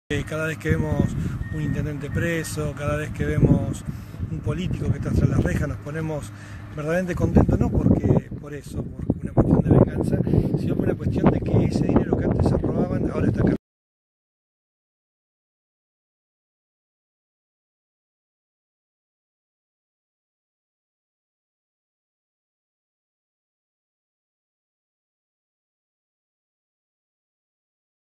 Aiola en las obras de la Autovía 7
El intendente de Chacabuco, Víctor Aiola, reconoció que "cada vez que vemos un político preso nos ponemos contentos"